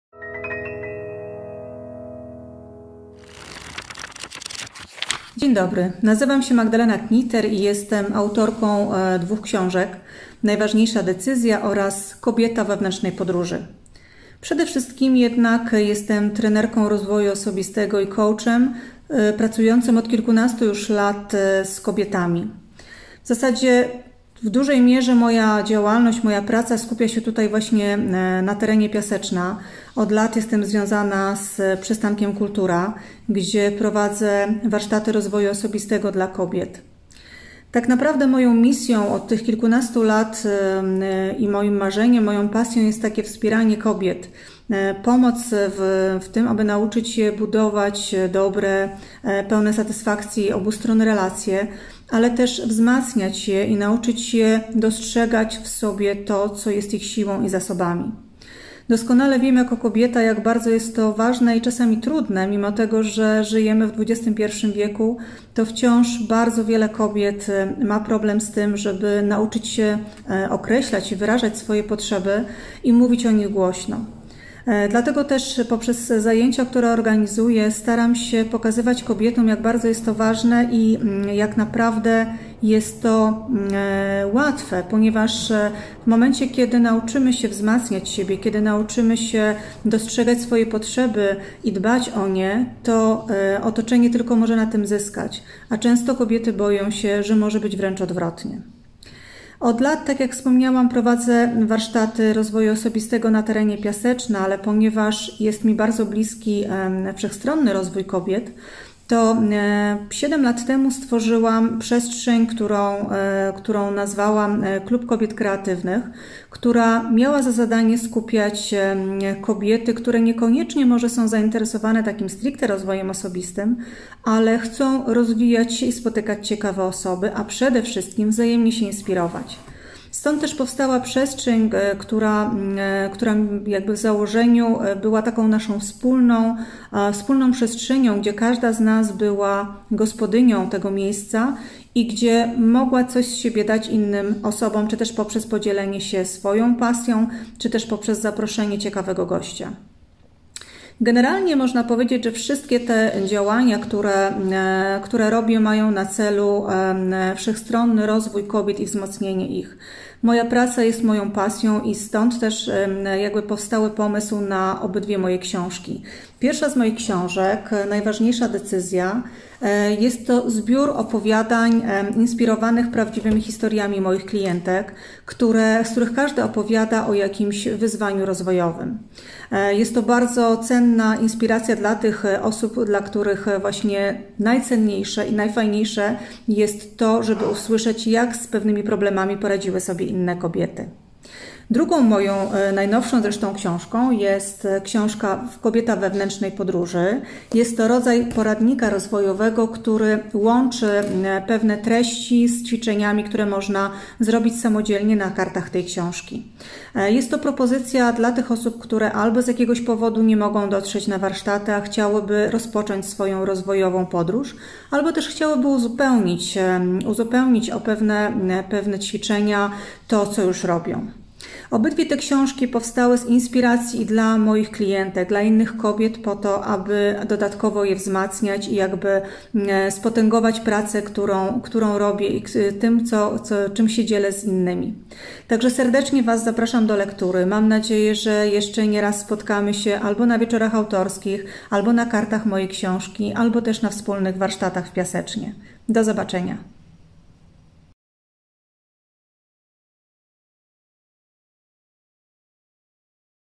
FESTIWAL // Magdalena Knitter
Dyplomowany life coach, trener rozwojowy i pedagog. Członek Polskiego Towarzystwa Psychologicznego.

Specjalizuje się w budowaniu relacji interpersonalnych oraz we wzmacnianiu poczucia własnej
wartości. Prowadzi warsztaty umiejętności interpersonalnych, grupy rozwojowe, coachingi indywidualne oraz wyjazdy z rozwojem dla kobiet do Toskanii.

Prowadzi również zajęcia dla pracowników Biur Karier, wykładowców oraz studentów w zakresie budowania efektywnych zespołów
oraz współpracy w zespole.

Autorka książek „Najważniejsza decyzja” oraz „Kobieta w wewnętrznej podróży”.

Festiwal organizowany jest w ramach projektu Festiwal Pięknej Książki „Piaseczno, Ludzie, Książki” realizowanego przez Bibliotekę Publiczną w Piasecznie oraz Fundację „Arka” im. Józefa Wilkonia we współpracy z Centrum Kultury w Piasecznie oraz dofinansowanego ze środków Narodowego Centrum Kultury w ramach programu Kultura – Interwencje 2020.